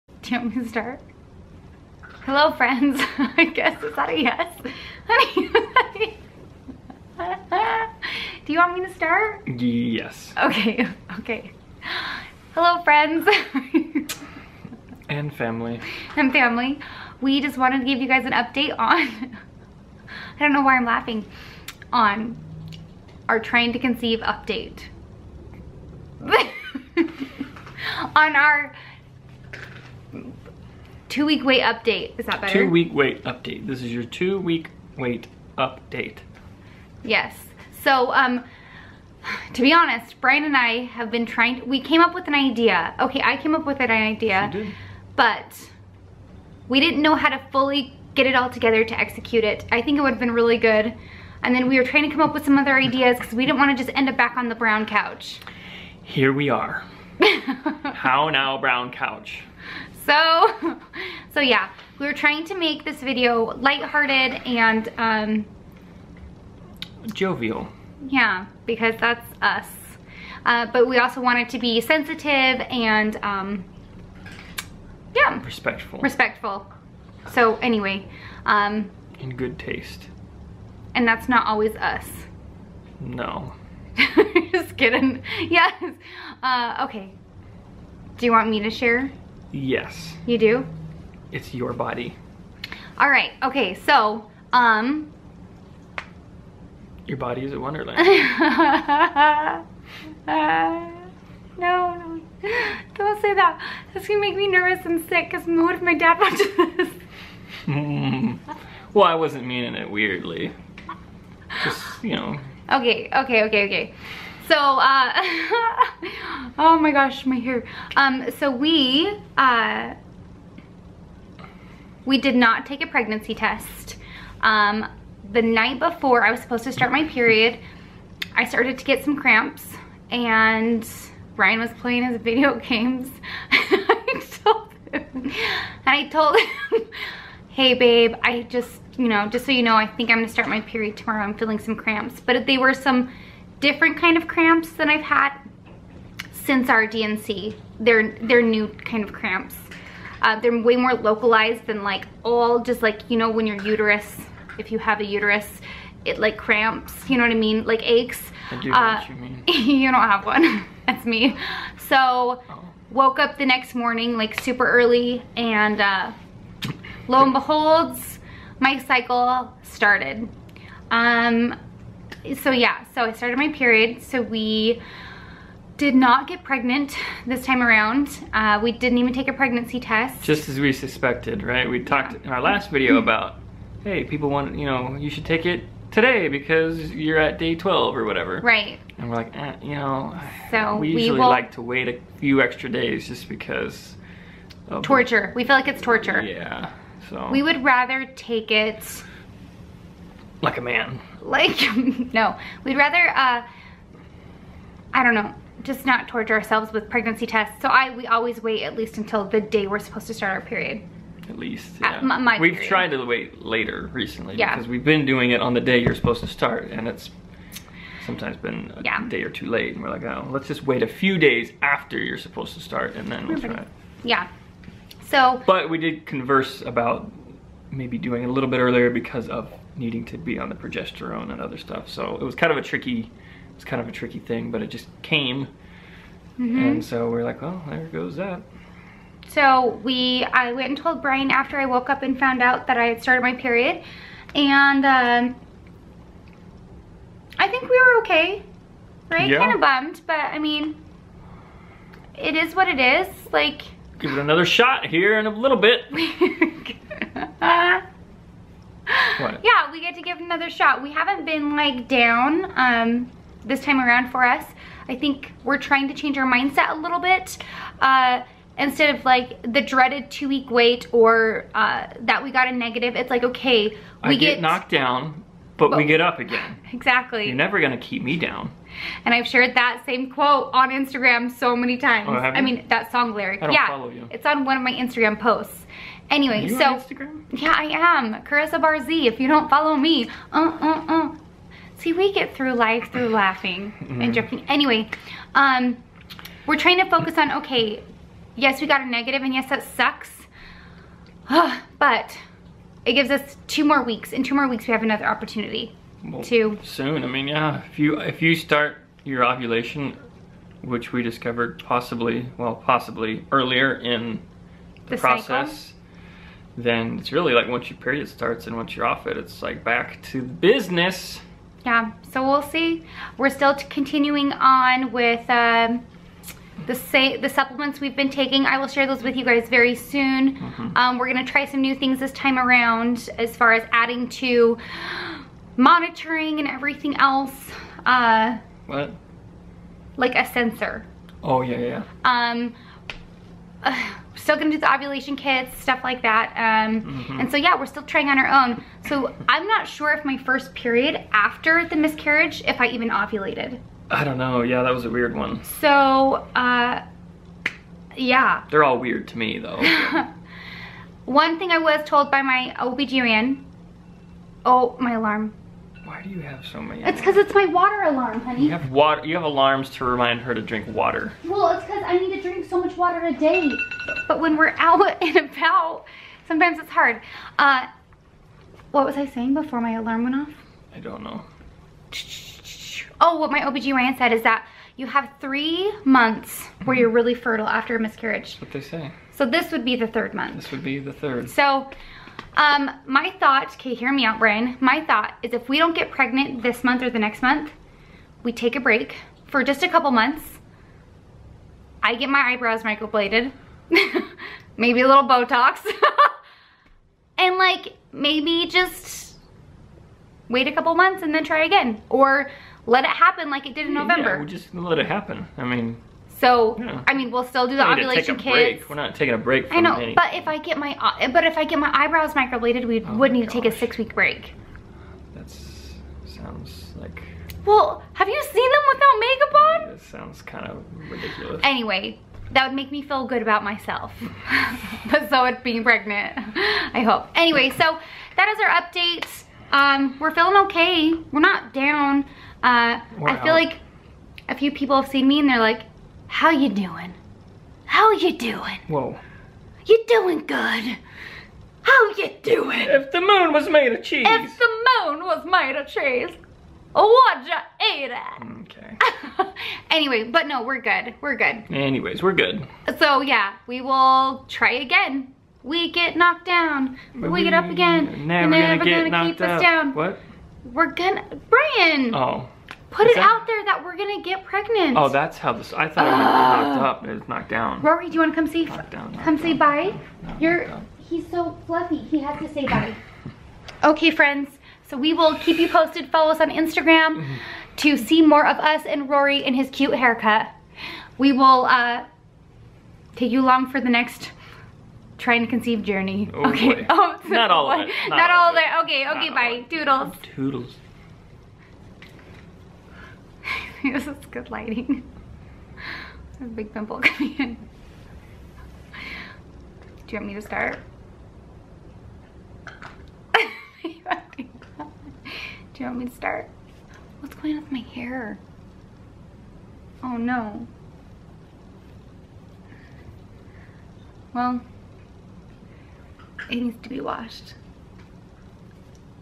Are We Pregnant? Two Week Wait Update
The wait is over and we are finally sharing if we are pregnant or not with our two week wait update. Brian and I had our second miscarriage in December and we have been trying to conceive (ttc) on our own since January. Unfortunately, we didn't get the best news but we still have good attitudes and hope that it will all work out. Thanks for the prayers and love!

Thank you for being a part of our fertility journey!
xo, Carissa and Brian

Liberty, MO 64069
TOODLES!!!